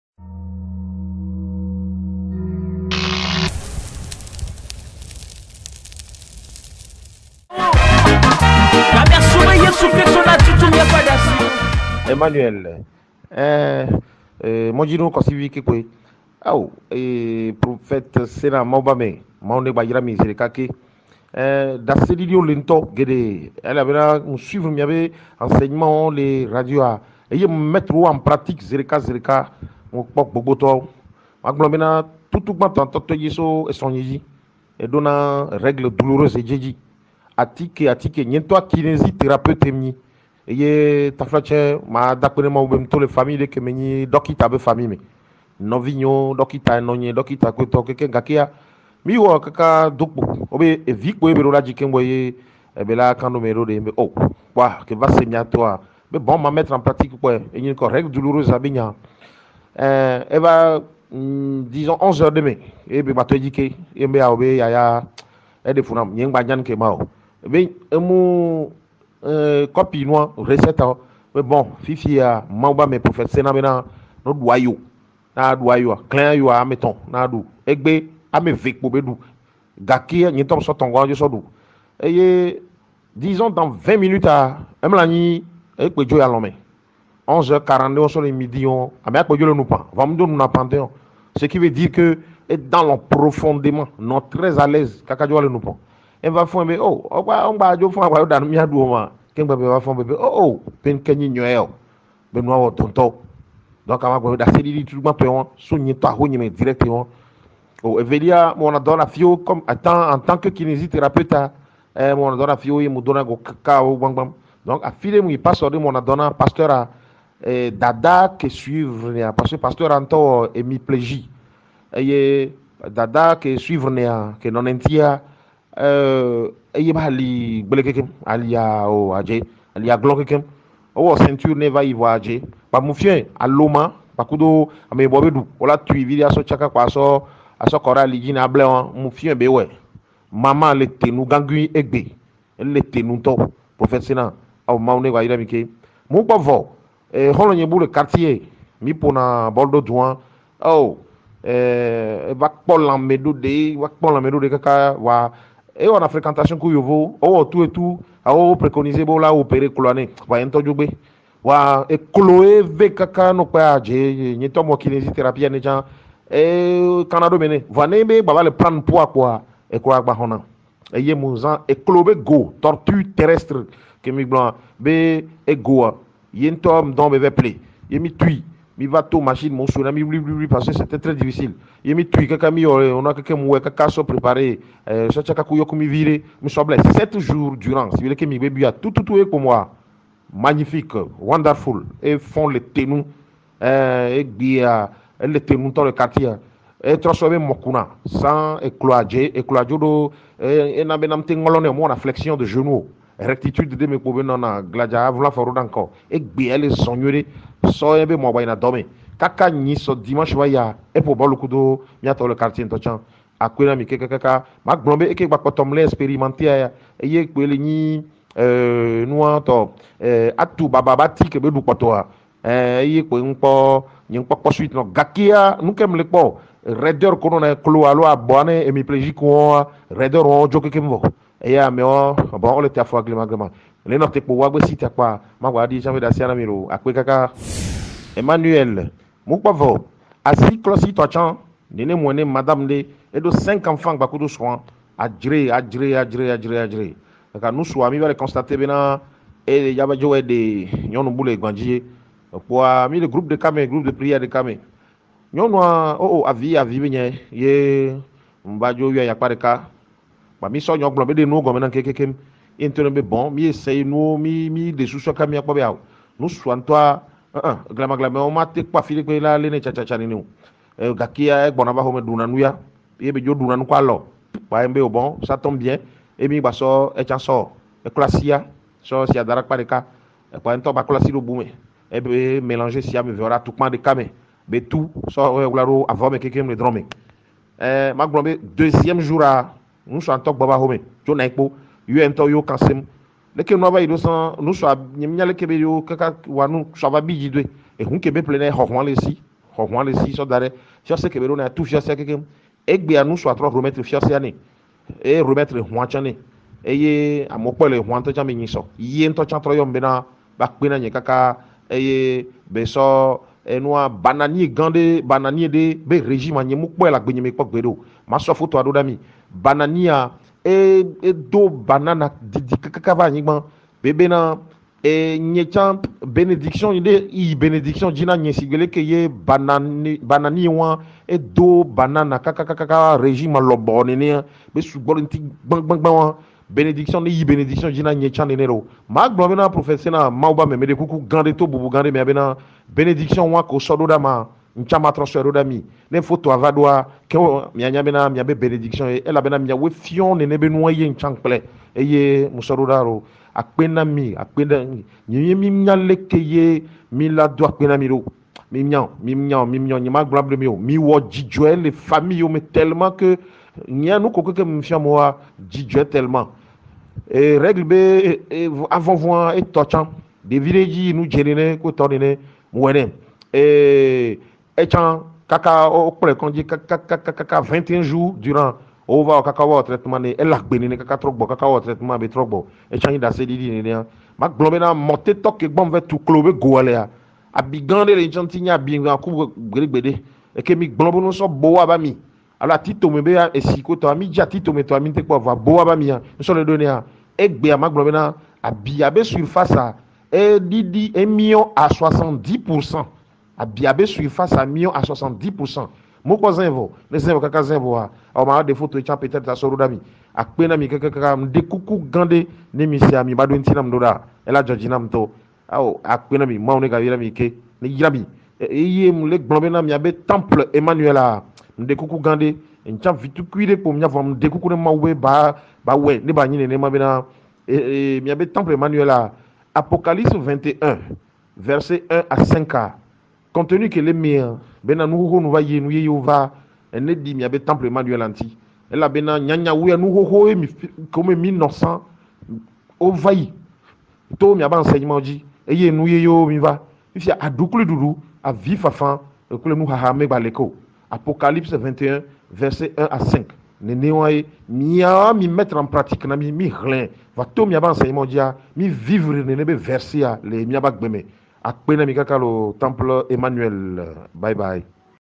TÉMOIGNAGE D'UN KINÉSITHÉRAPEUTE PAR WHATSAPP_Prophète SENA (TEMPLE EMMANUEL)
Témoignage d’un kinésithérapeute par whatsapp sur l’enseignement du Prophète SENA (TEMPLE EMMANUEL)...
SI LE PROPHÈTE SENA A APPORTE QUELQUES CHOSES DANS VOTRE VIE, FAITES NOUS PARVENIR VOS TÉMOIGNAGES PAR WHATSAPP AU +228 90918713